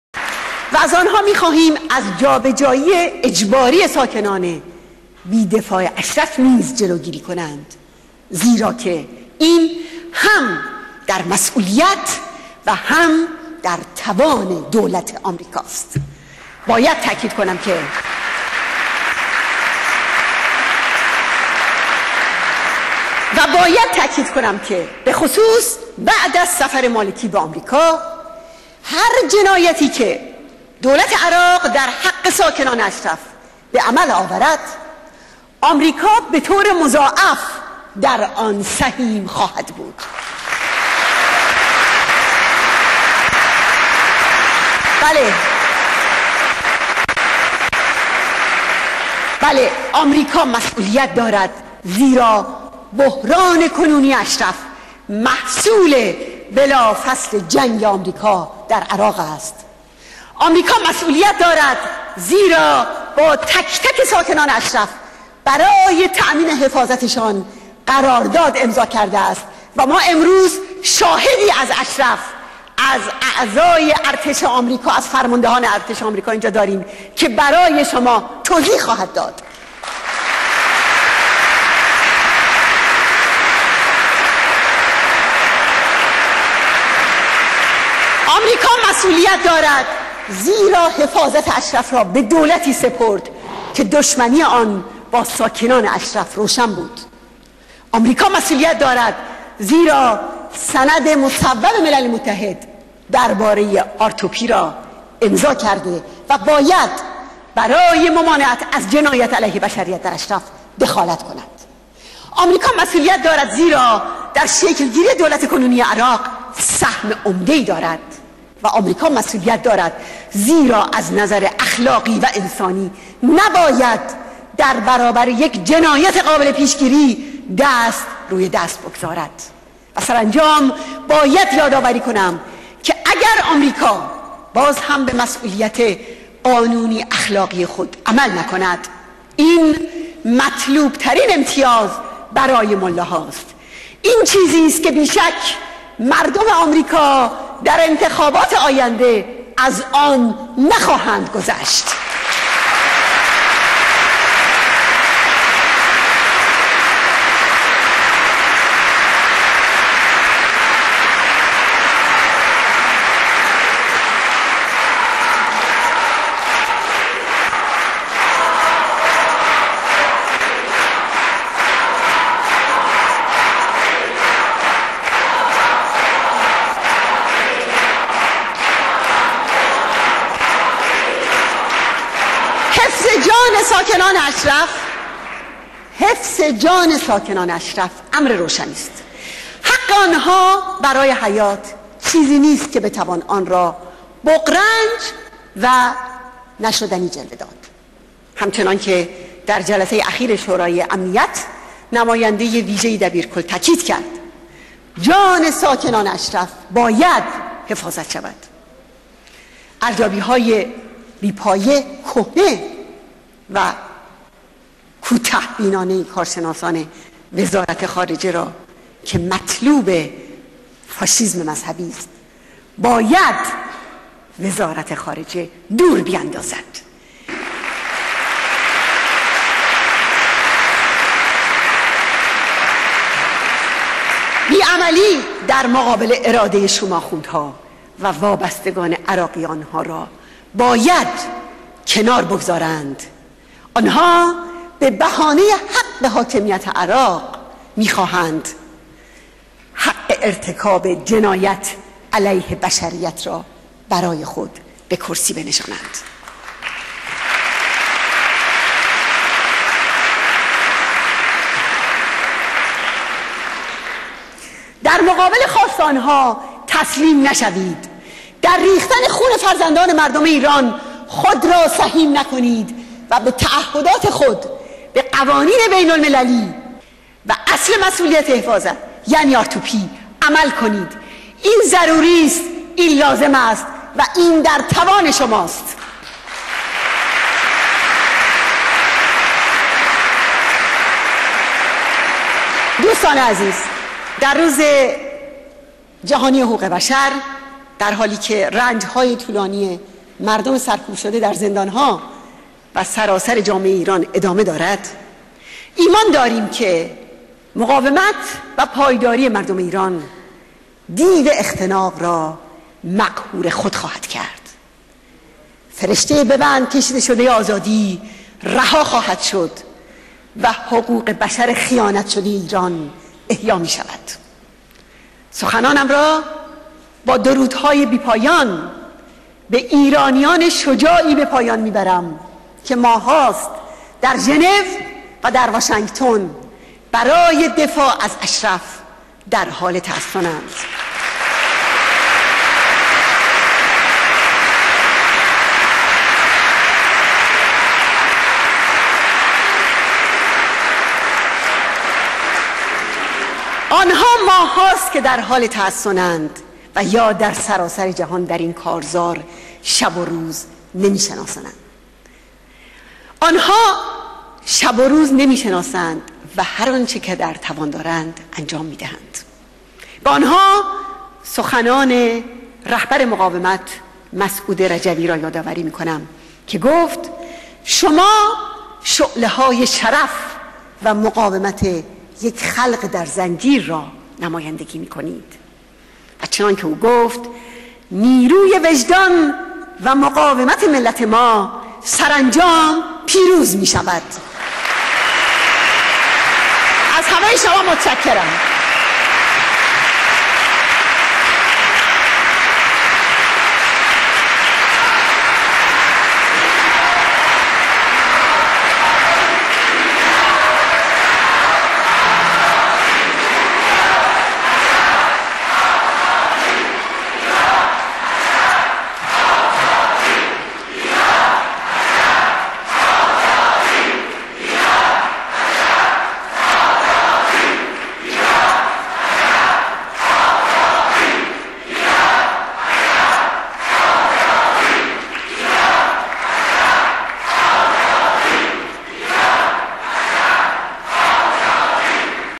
كنفرانس بين المللي پاريس- مريم رجوي - روزحقوق بشر3
كنفرانس بين المللي پاريس باحضورخانم مريم رجوي,حفاظت از مجاهدان اشرف , لغو ضرب الاجل سركوبگرانه
روز جهاني حقوق بشر-10 دسامبر-19 آذر 1390
خانم رجوي: بايد تاكيد كنم كه بعد از سفر مالكي به آمريكا هر جنايتي كه  دولت عراق در حق ساكنان اشرف به عمل آورد , آمريكا بطور مضاعف در آن سهيم خواهد بود. آمريكا مسئوليت دارد زيرا بحران كنوني  اشرف محصول بلافصل جنگ آمريكا در عراق است.
خانم رجوي افزود امروز در روز جهاني حقوق بشر صداي پروفسور دكتر  كاظم رجوي شهيد بزرگ حقوق بشر ايران را دوباره مي شنويم كه گفت ما تاريخ حقوق بشر حقيقي ايران را با خون خود مي نويسيم .